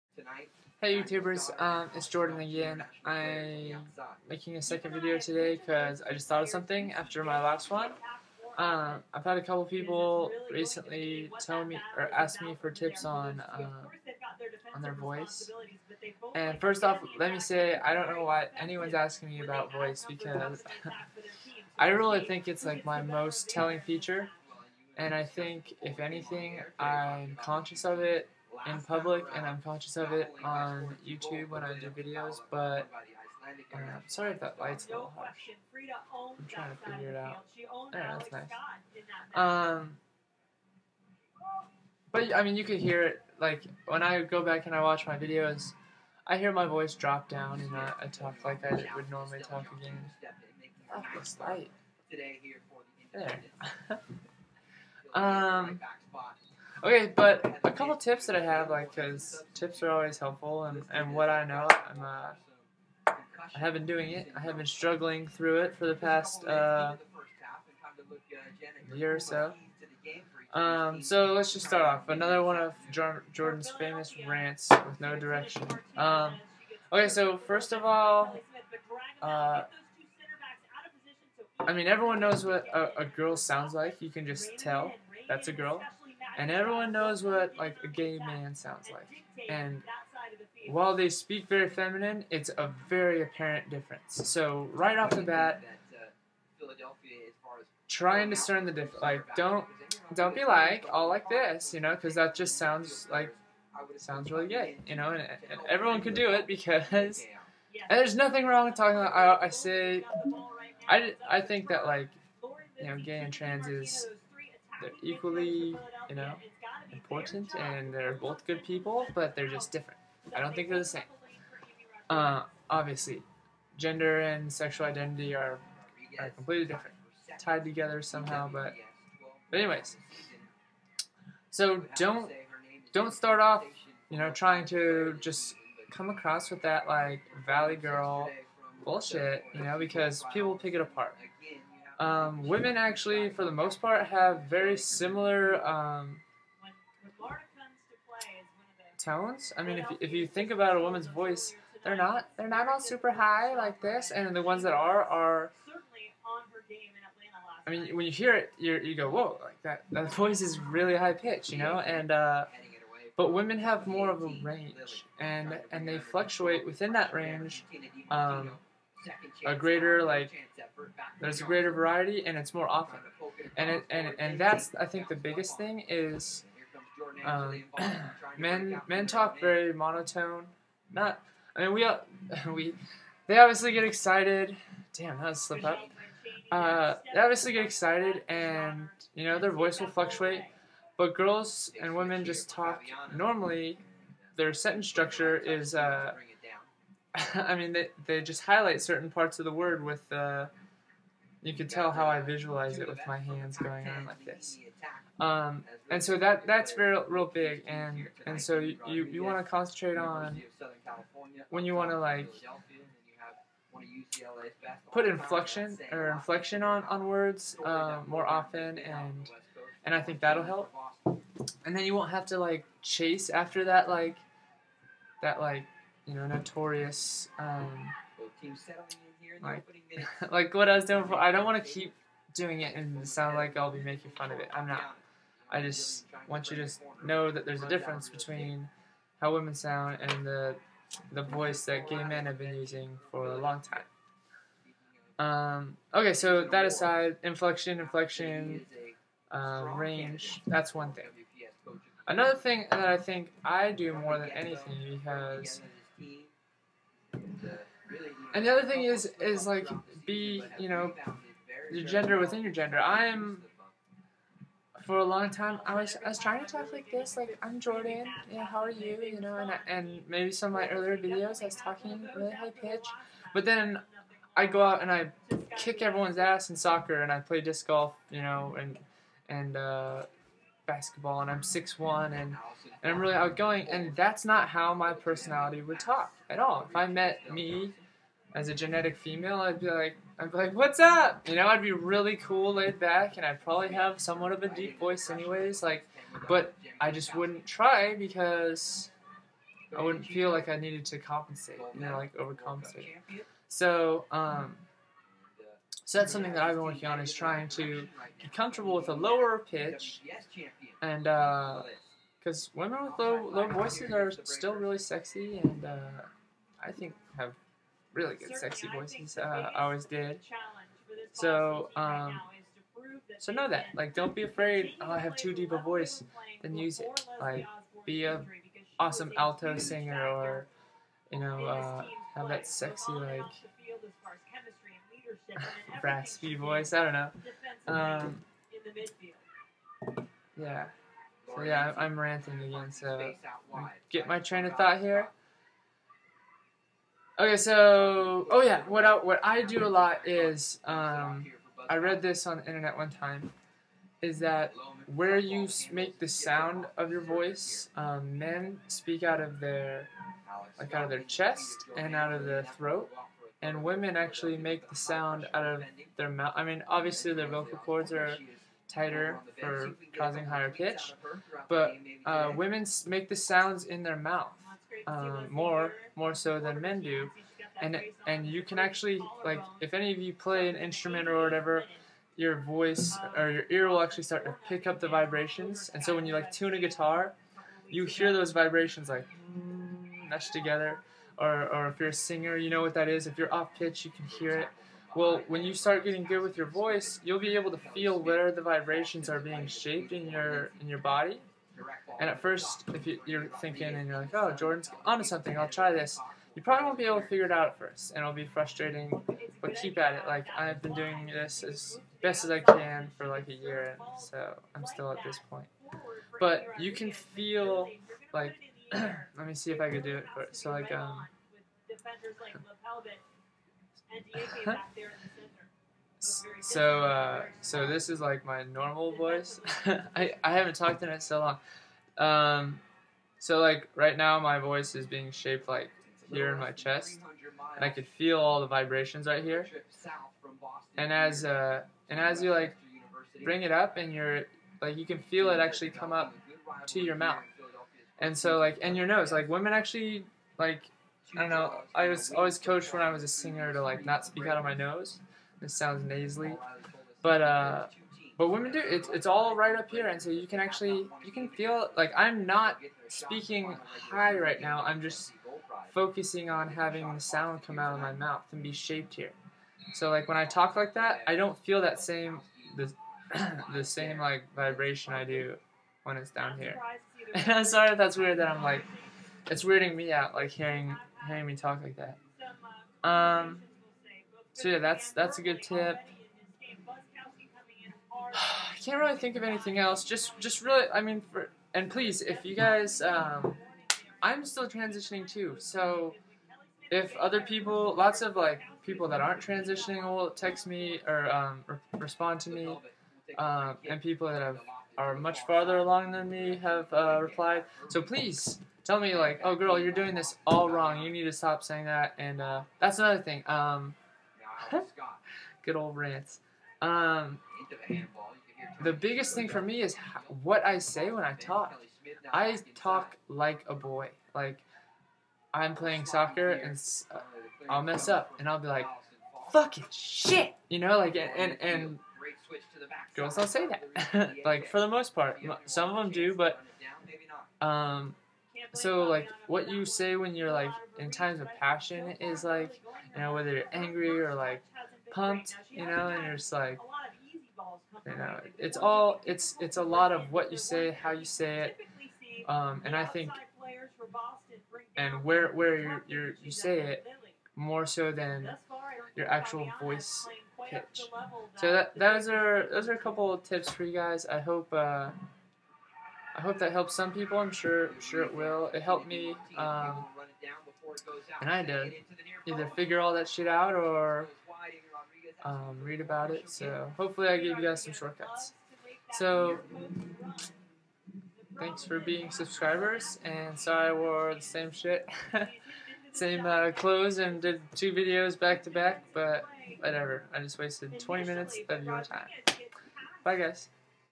Tips on voice stuff
So i just thought of this. Lately people have been asking about voice stuff. Here are some tips.
-inflection, pace
-where you shape the sounds (stomache vs. mouth/nose)
-what you are saying